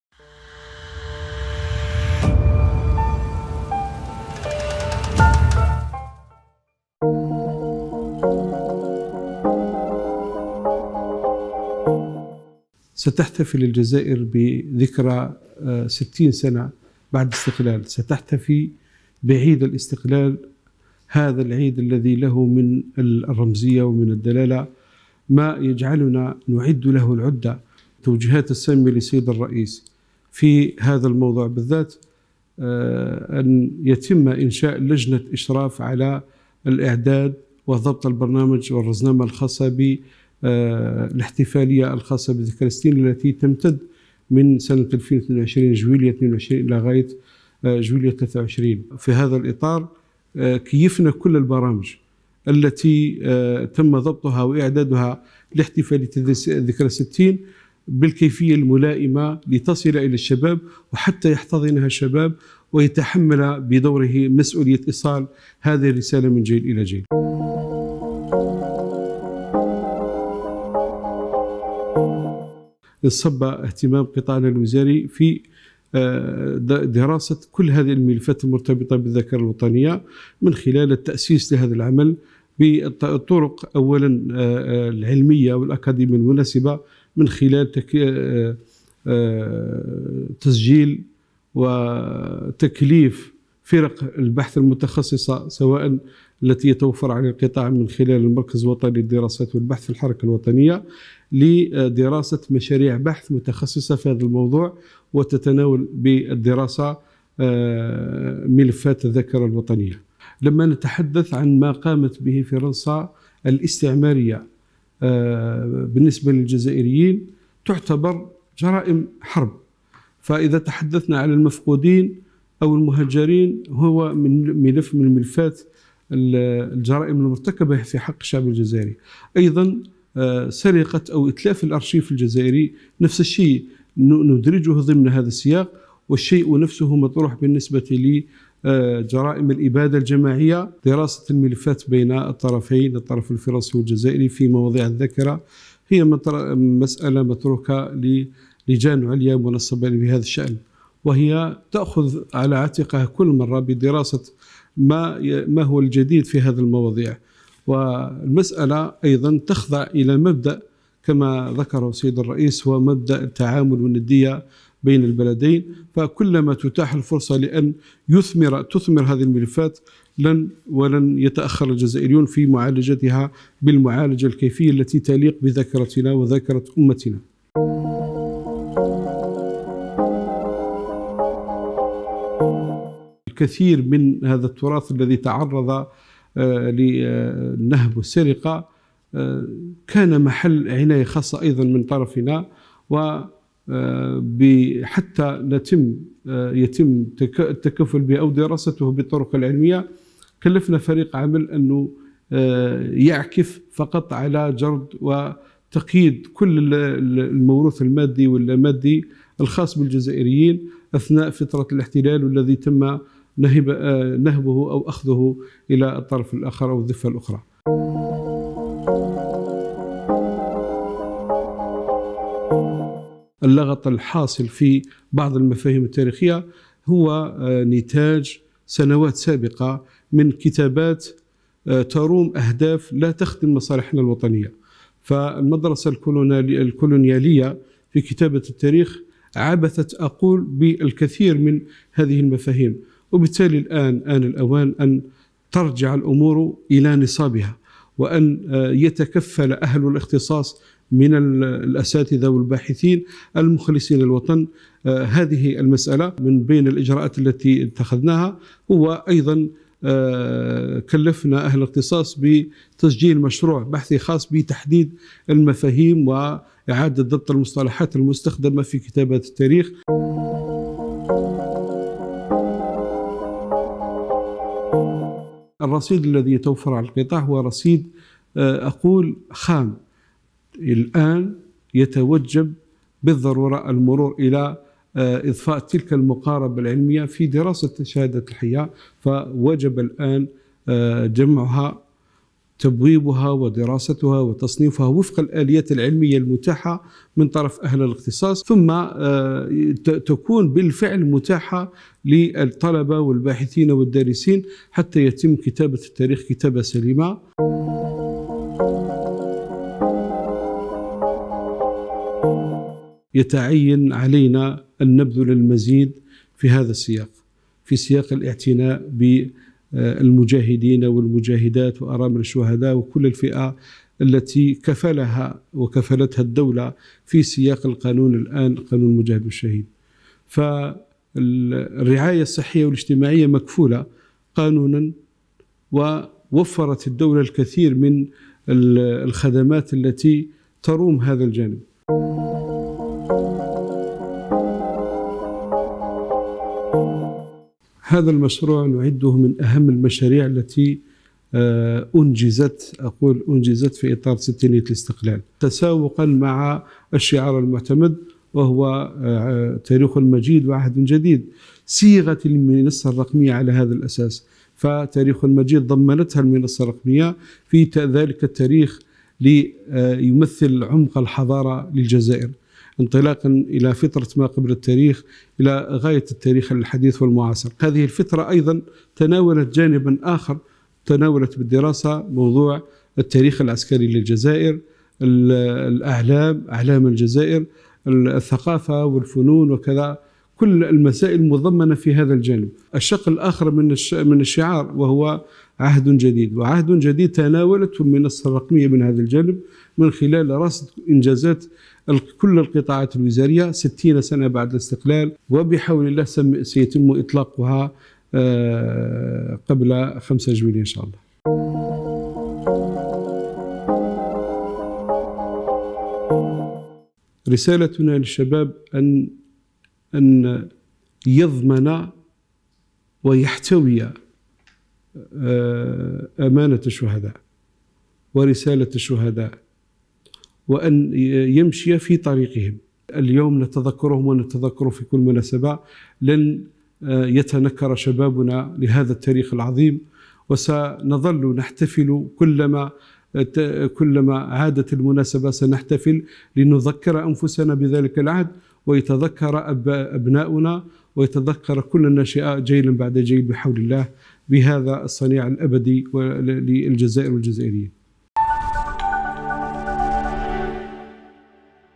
الذكرى الـ 60 للاستقلال: وزير المجاهدين يدعو الشباب إلى احتضان رسالة نوفمبر وضمان استمراريتها
الجزائر (وأج)- دعا وزير المجاهدين وذوي الحقوق العيد ربيقة، الشباب الجزائري الى احتضان رسالة نوفمبر التي تركها الشهداء والمجاهدون وضمان استمراريتها وإيصالها إلى الأجيال القادمة، مبرزا في حديث خص به وأج عشية الاحتفالات المخلدة للذكرى الـ 60 لاسترجاع السيادة الوطنية، أن الجزائر تعيش في هذه الفترة زخما معتبرا من الأحداث المتعاقبة تتخللها الاحتفالات المخلدة للاستقلال وافتكاك حريتنا من الاستعمار الغاشم.